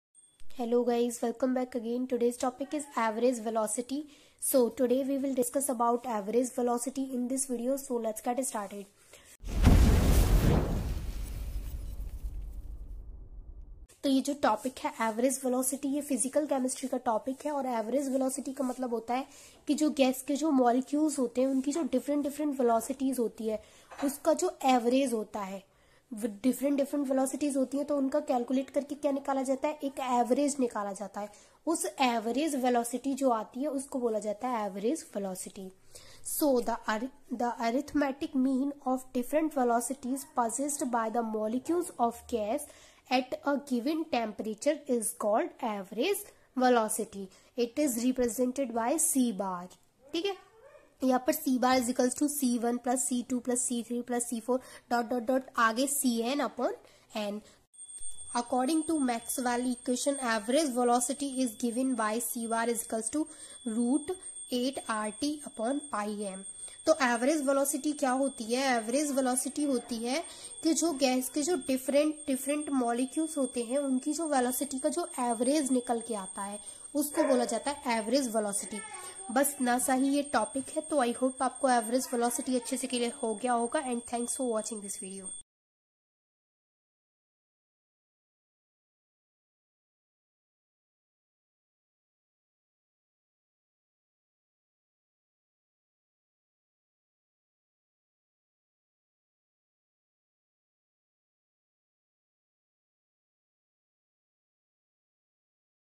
Average velocity | Average velocity formula | Average velocity bsc 1st year
We will learn about "Average velocity" In this lacture.

Today we will learn about "Average velocity" in this lacture.
If you have any doubt you can ask in below comment box.

Social links -
coming soon